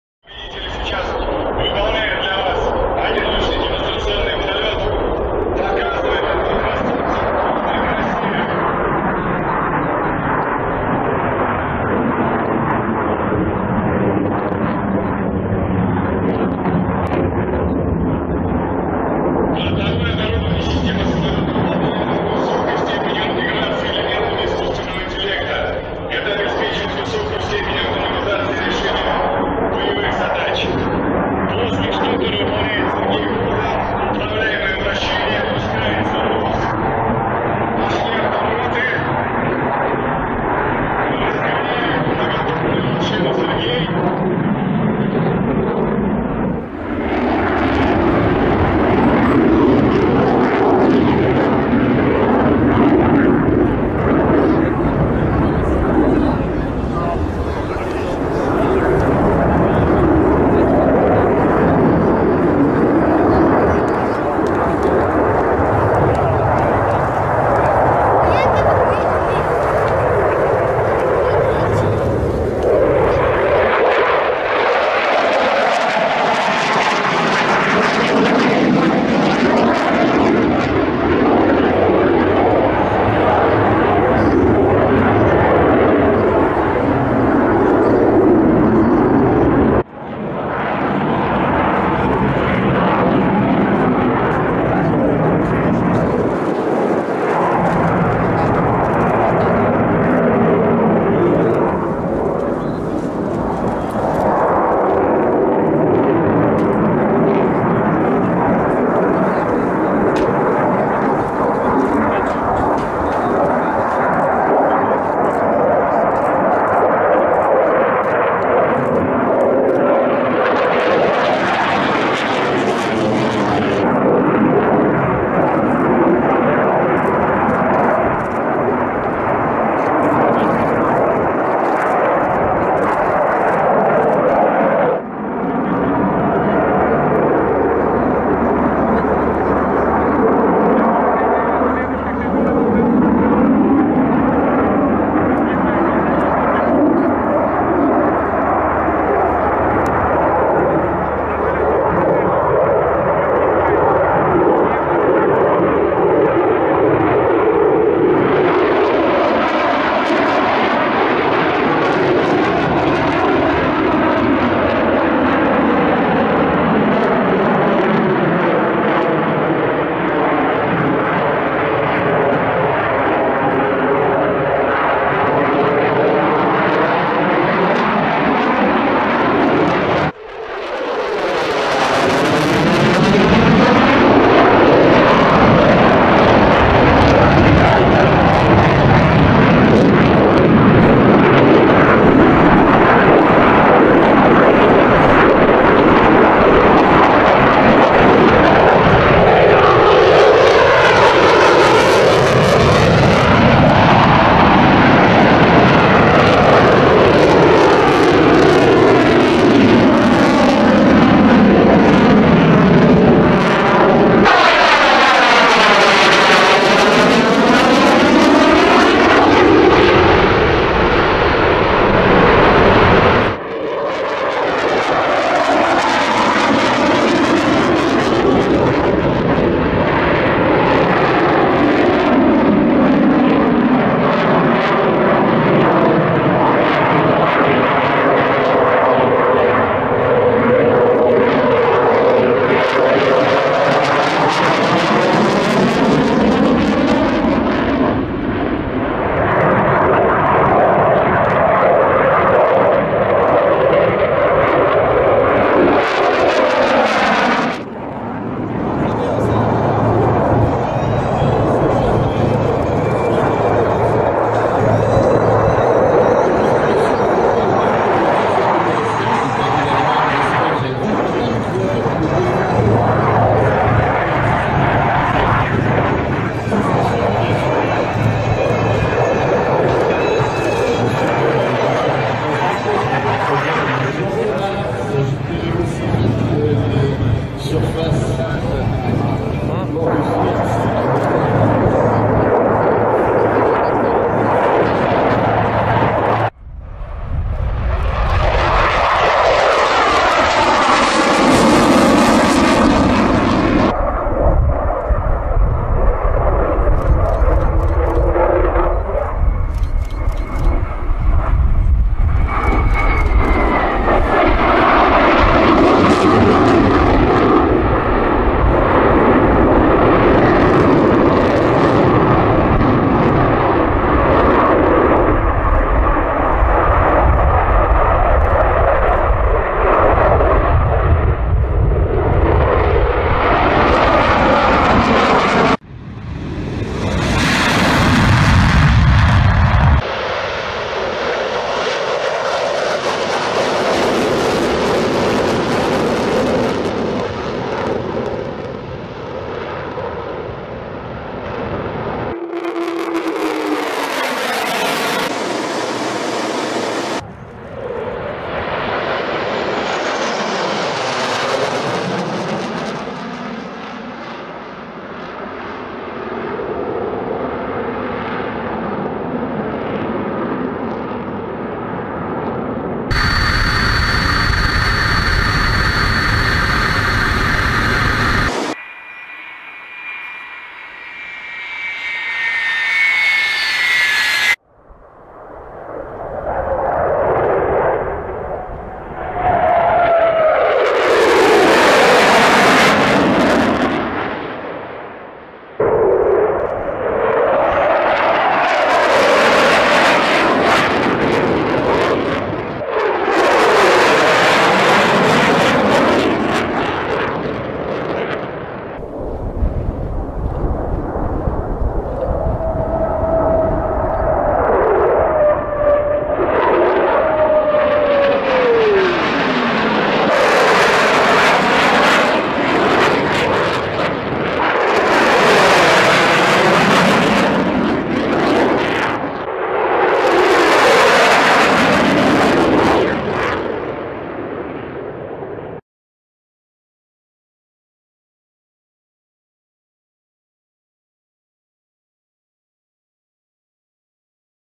Fastest Fighter Jets 2019
A fighter aircraft is a military aircraft designed primarily for air-to-air combat against other aircraft, as opposed to bombers and attack aircraft, whose main mission is to attack ground targets. The hallmarks of a fighter are its speed, maneuverability, and small size relative to other combat aircraft.